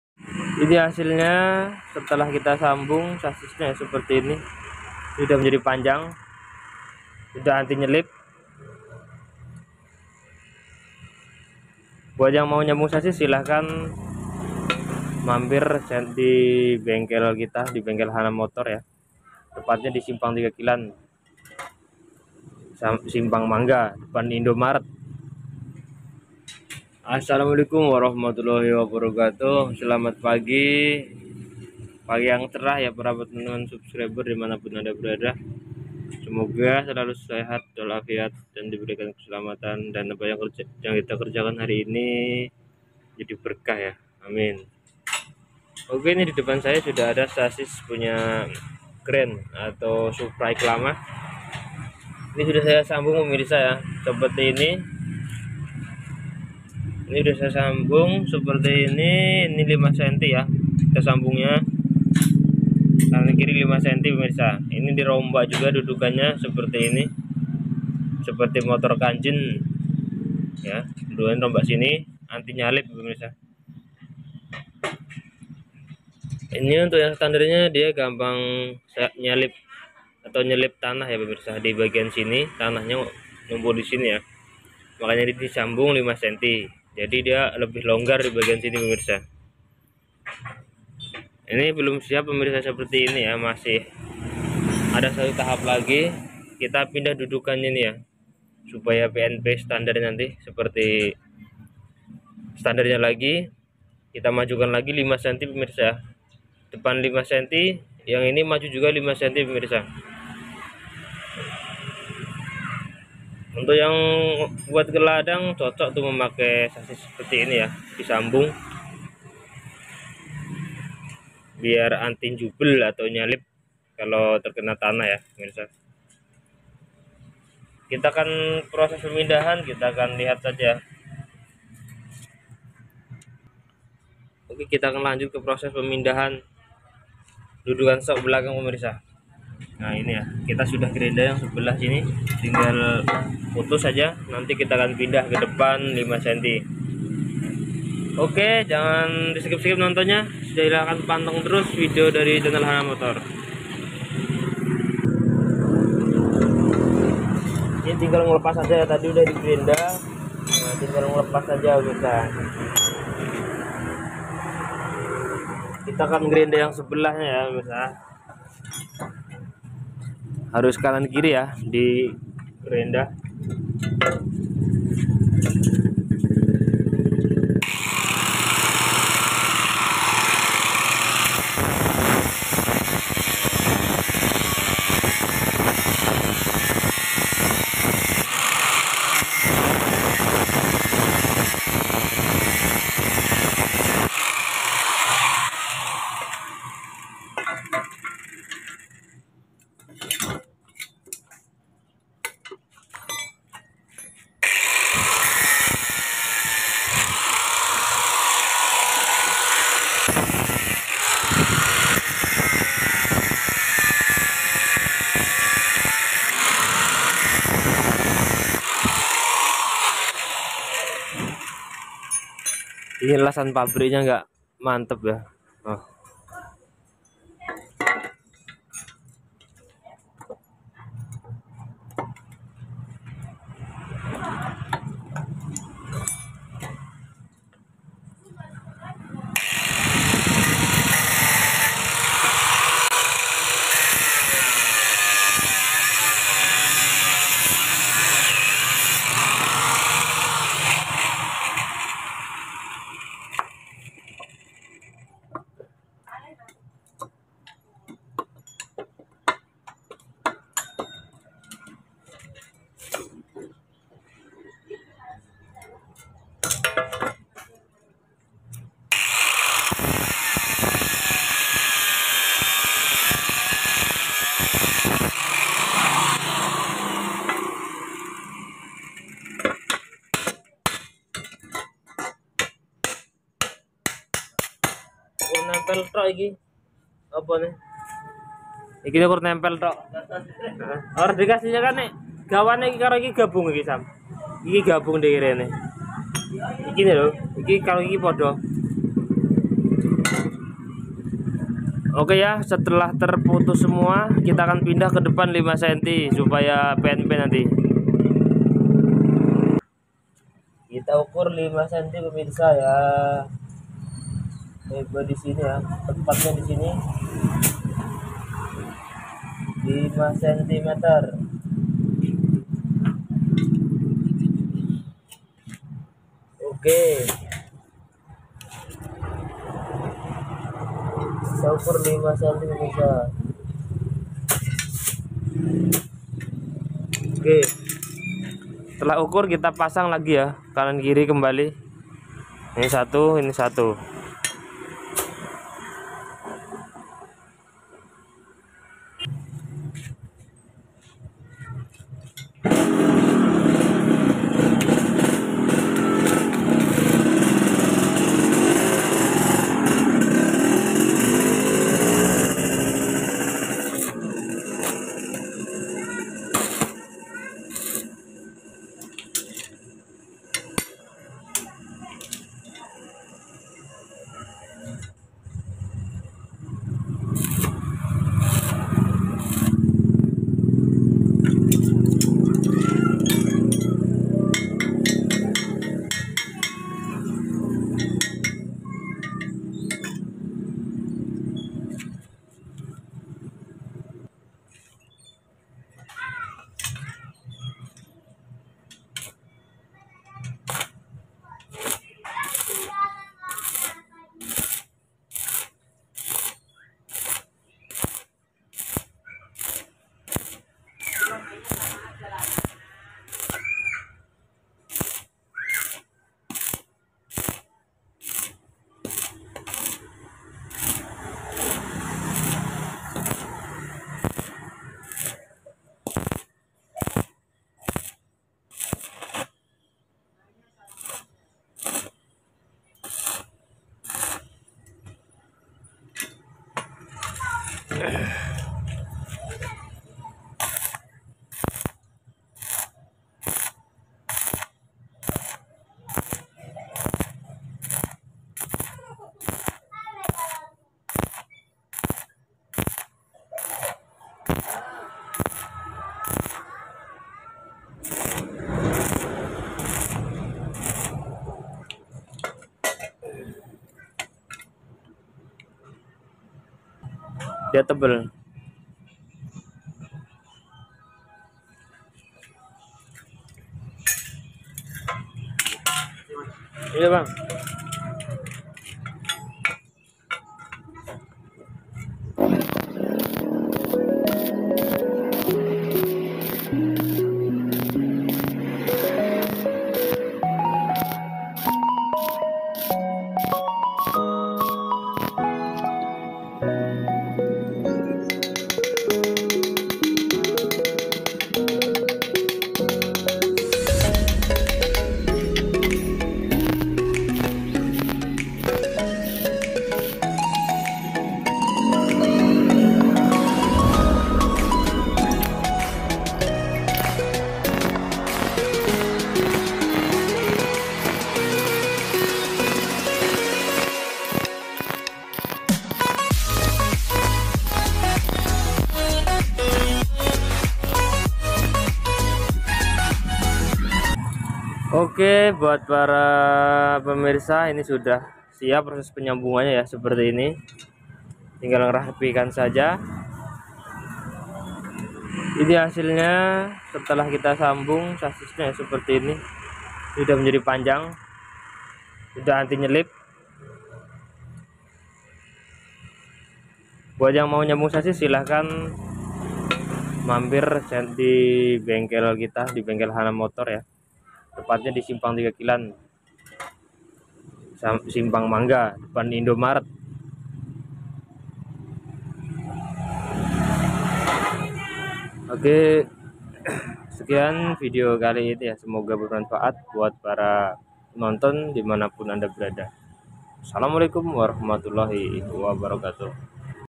cara sambung sasis honda supra x sangat mudah
biasanya sasis  seperti ini sering dipake pada motor khusus langsir sawit
motor tidak mudah jamping, dan tanah tidak nyangkut di sasis#video